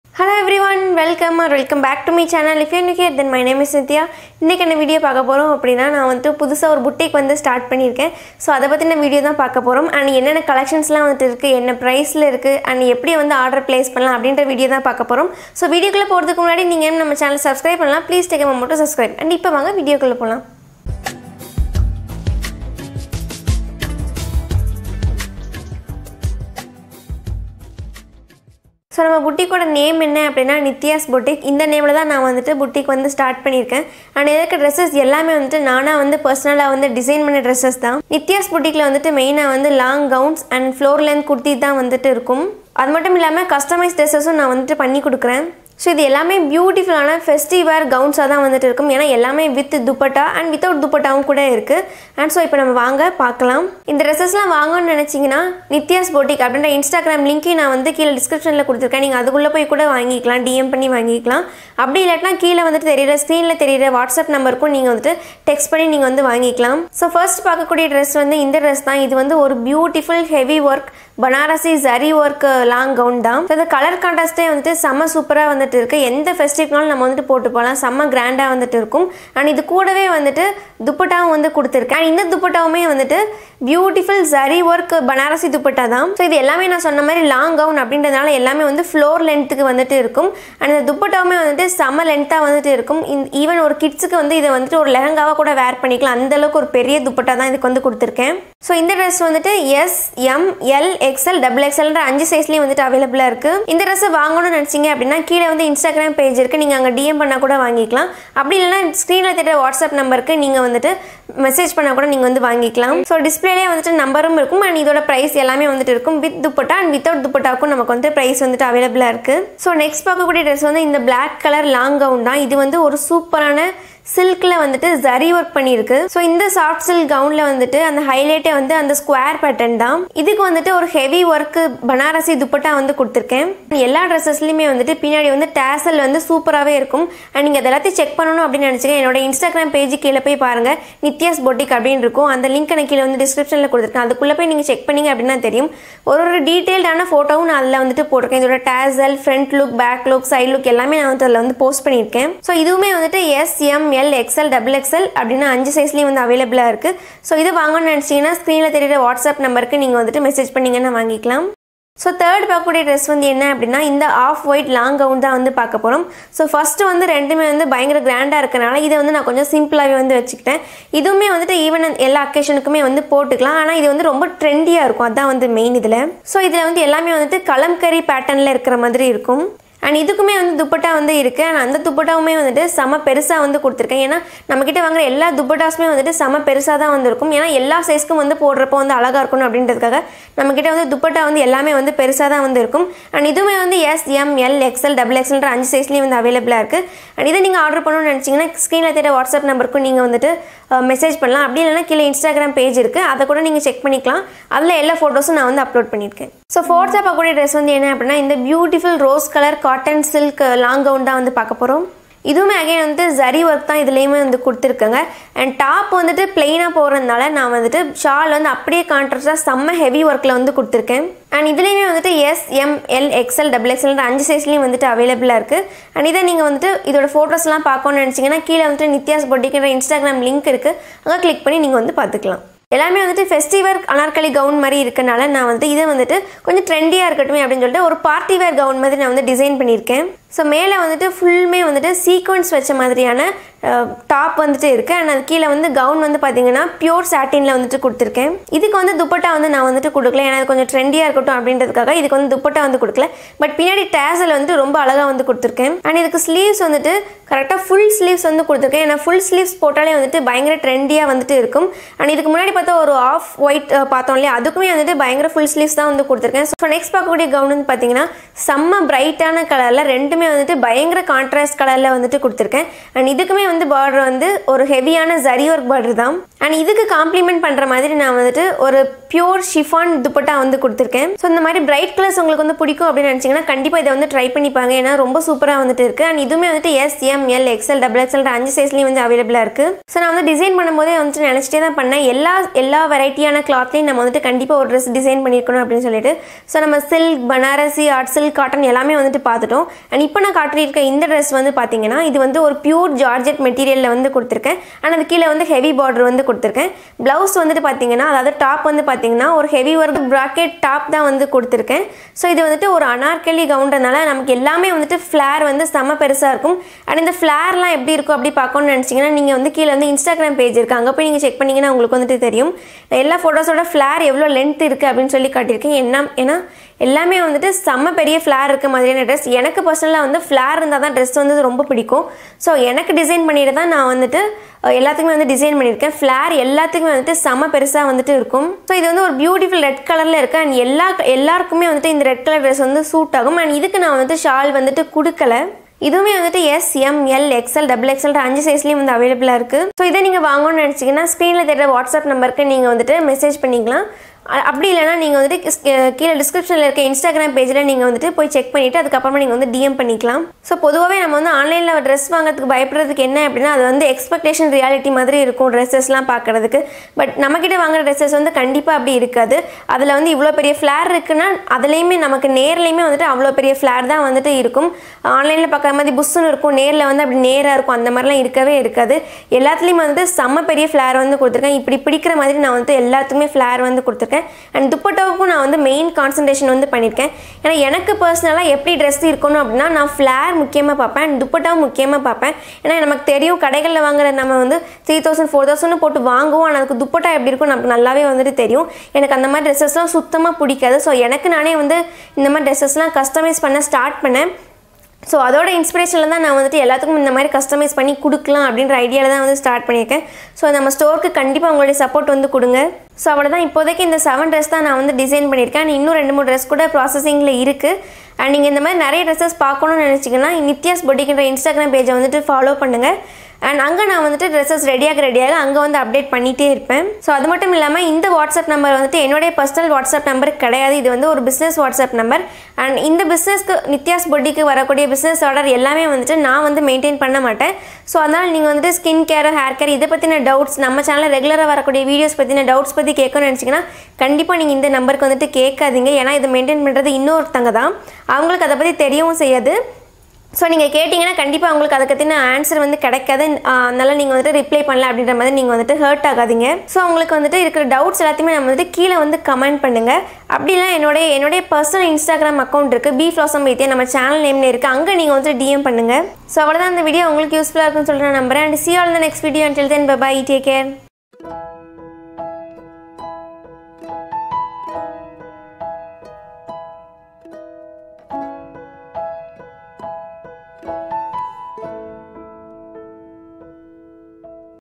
Customized floor length dress at affordable price🔥 | Nithyas boutique my new journey begins😍
Hello everyone,
  In this video i have shared my customized floor length dress collection which were designed by myself and also i am happy to announce that these dresses are available at Nithyas boutique(my new store). You can buy these dresses through Nithyas boutique instagram page or whatsapp to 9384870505.

Love❤
Nithya